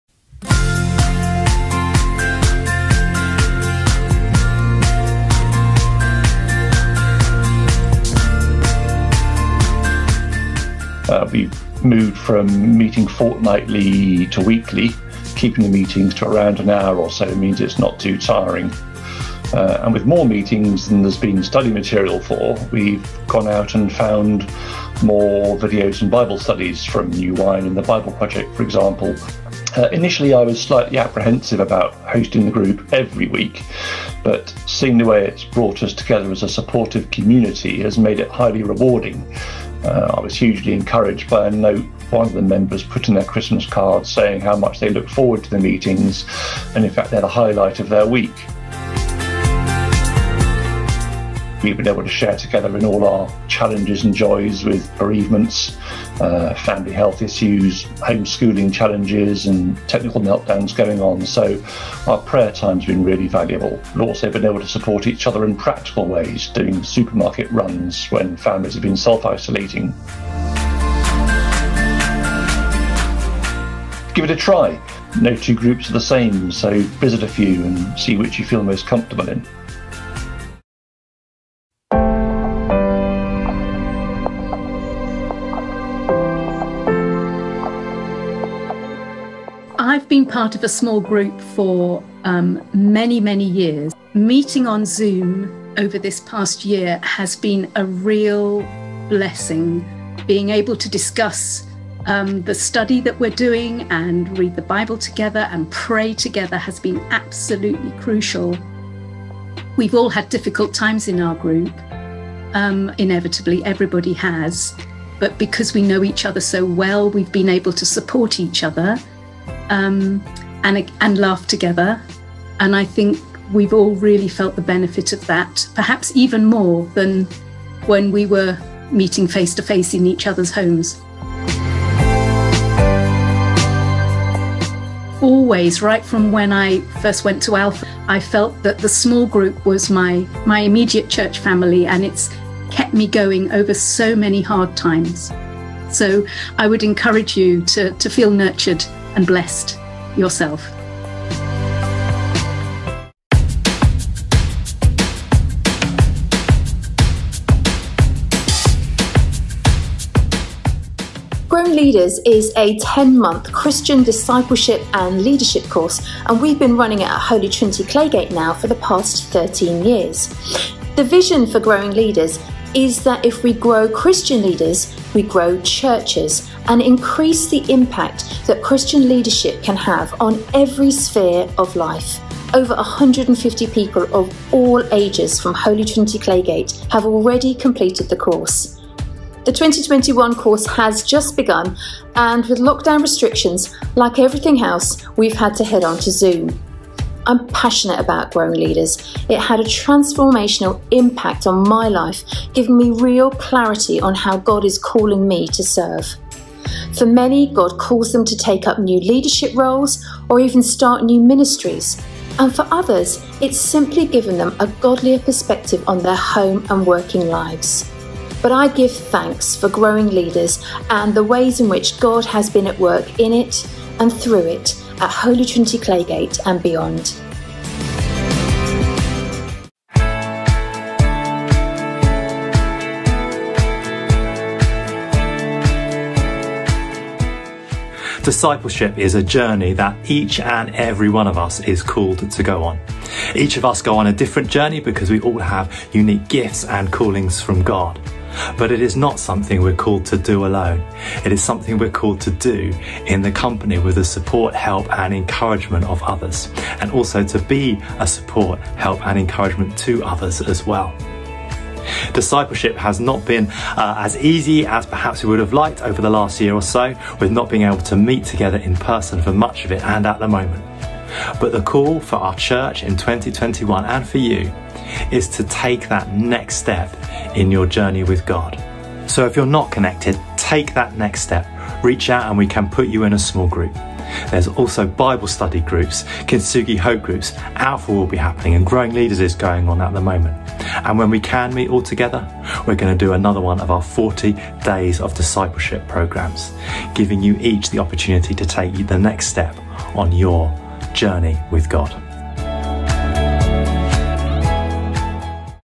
One Minute Messages: Discipleship
In this video we hear how discipleship continues to bless those taking part and what the vision for 2021 looks like, in supporting and encouraging us all in our journey with God.